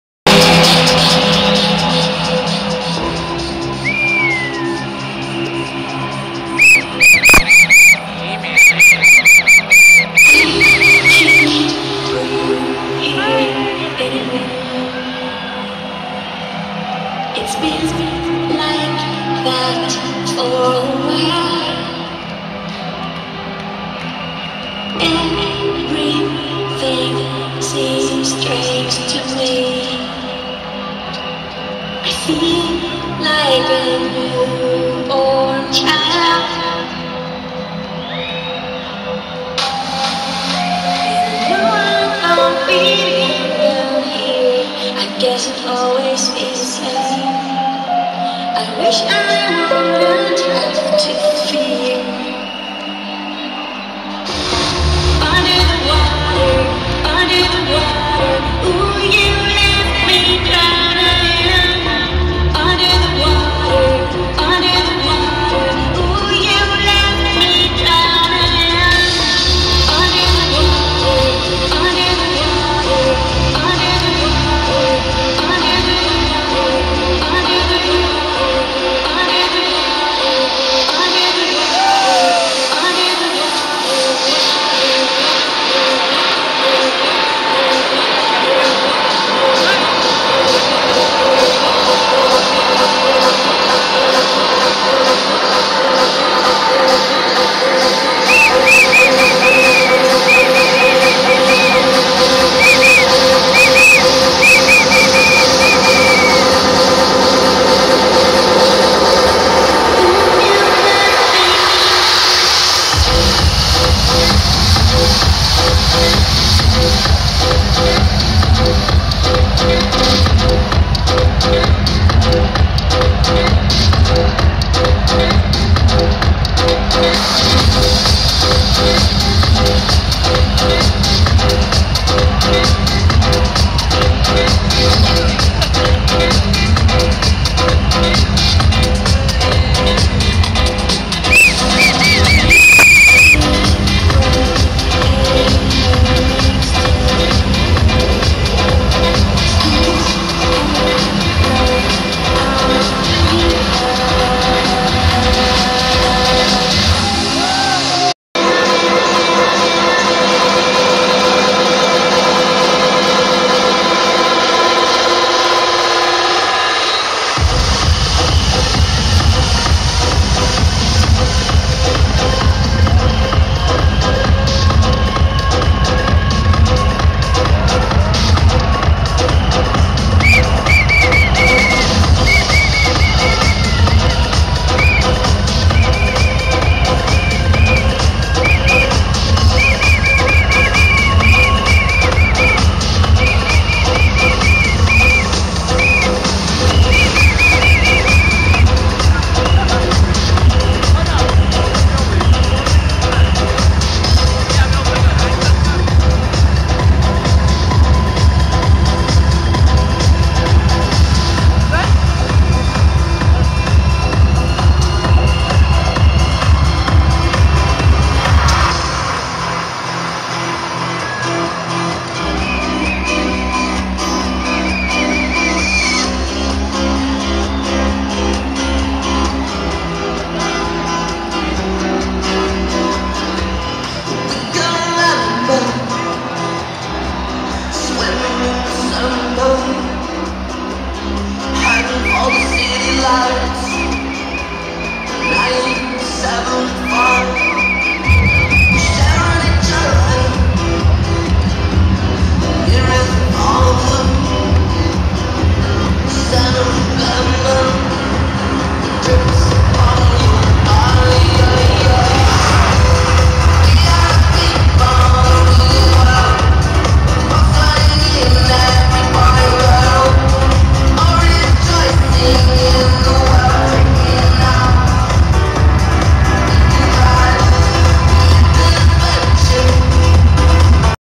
Tiesto at the SECC Glasgow July 2009
DJ Tiesto doing a set at the SECC in Glasgow in summer july 2009!MINTED!